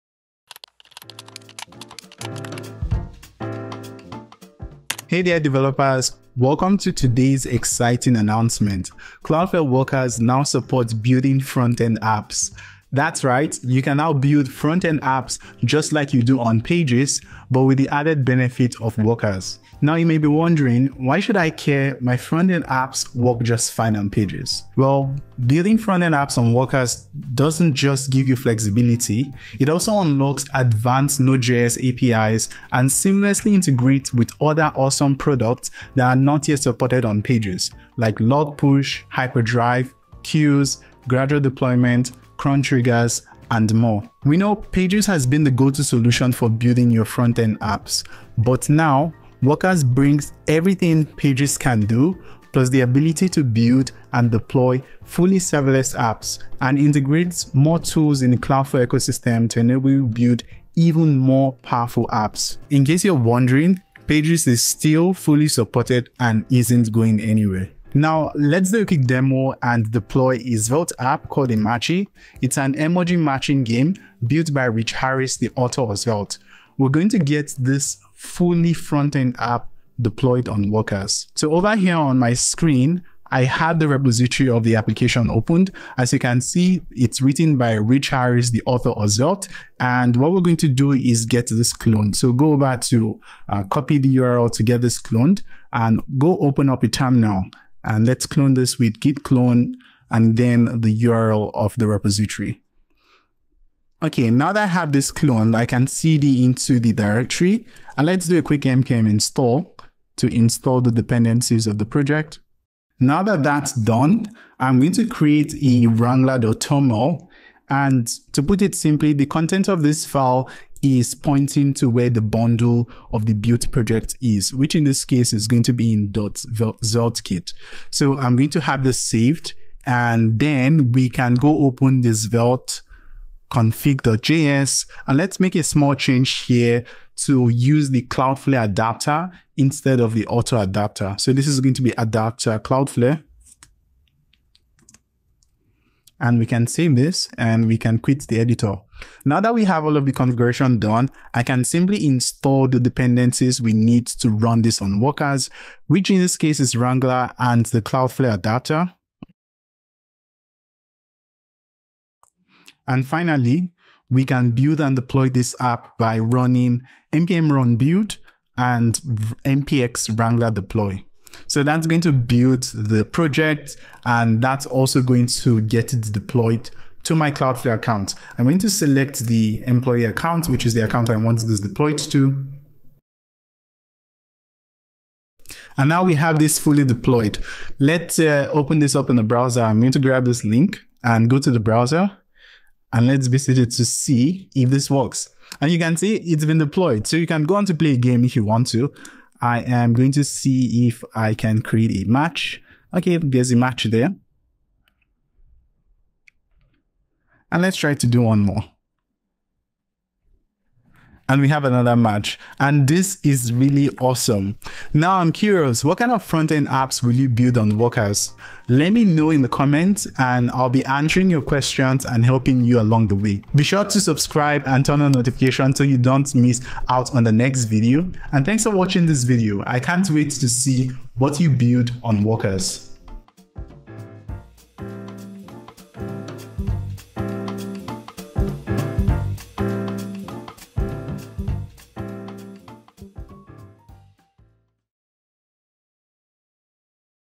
Building Front-End Applications: Now Supported by Cloudflare Workers
Hey there developers! Welcome to today's exciting announcement. Cloudflare Workers now
supports building Frontend Apps. That's right! You can now build Frontend Apps
just like you do on Pages, but with the added benefit of Workers.

Chapters:
00:00 Intro
01:06 Front end application demo
02:43 Build and deploy your app
03:03 Test your front end app
03:34 Conclusion

Host: Confidence Okoghenun